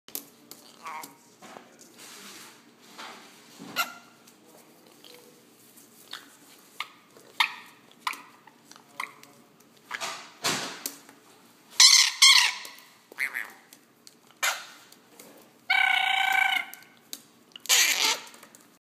Rainbow Lorikeet Playing
Georgie wrestles a little with myself and his favorite pen :)